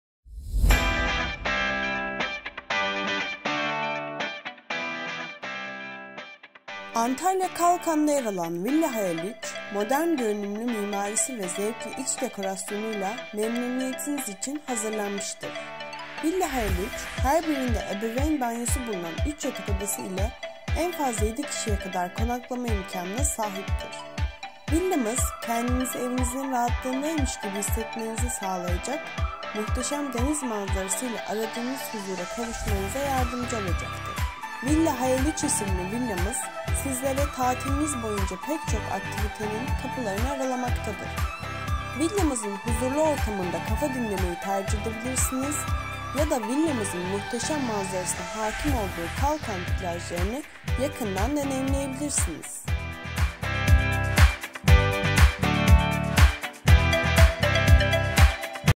Kalabalık Aileler İçin Kiralık Villa – Villa Hayal 3 |TNK Tatilnekolay
Antalya Kalkan’da yer alan Villa Hayal 3 modern görünümlü mimarisi ve zevkli iç dekorasyonuyla memnuniyetiniz için hazırlanmıştır.
Villa Hayal 3 her birinde ebeveyn banyosu bulunan 3 yatak odası ile en fazla 7 kişiye kadar konaklama imkanına sahiptir.
Villamız  kendinizi evinizin rahatlığındaymış gibi hissetmenizi sağlayacak, muhteşem deniz manzarası ile aradığınız huzura kavuşmanıza yardımcı olacaktır.
Villa Hayal 3 isimli villamız sizlere tatiliniz boyunca pek çok aktivitenin kapılarını aralamaktadır.
Villamızın huzurlu ortamında kafa dinlemeyi tercih edebilirsiniz ya da villamızın muhteşem manzarasına hakim olduğu Kalkan plajlarını yakından deneyimleyebilirsiniz.